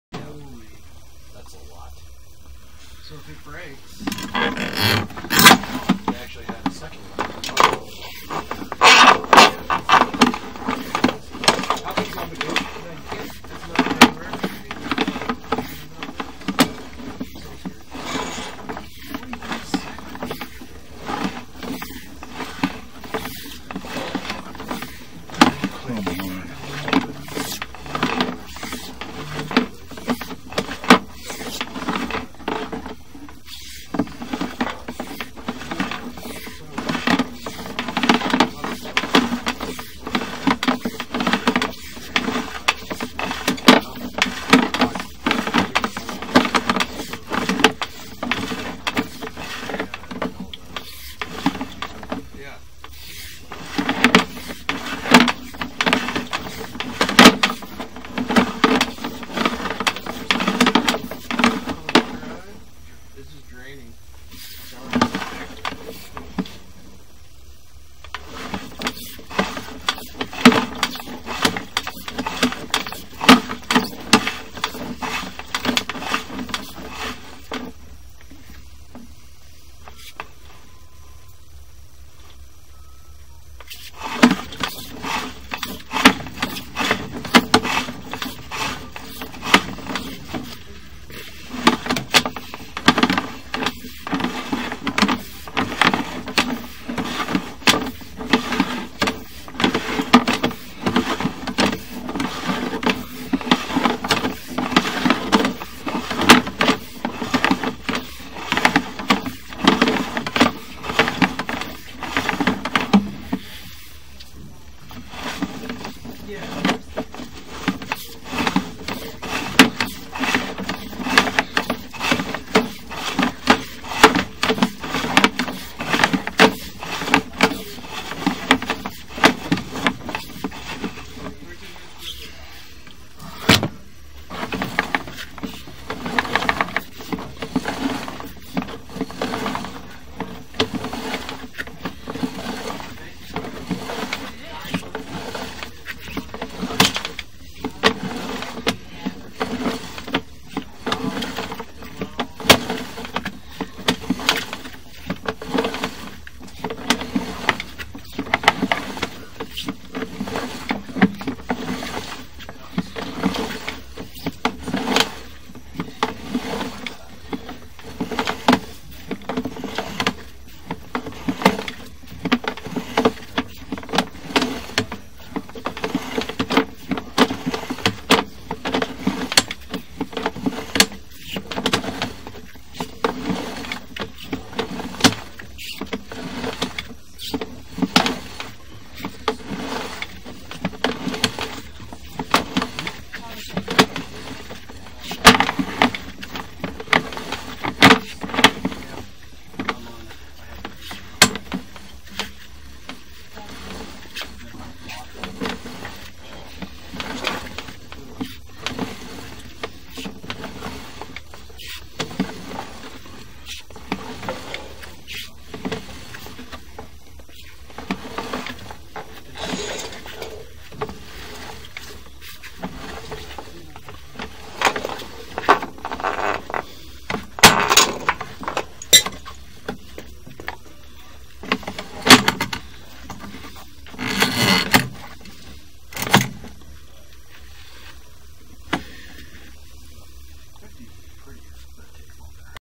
Sewer Scope video for 9120 Melbourne Dr. Colorado Springs, Co. 80920
Video recorded by Scott Whibeck. Hometeam of Colorado Springs. 2/10/2022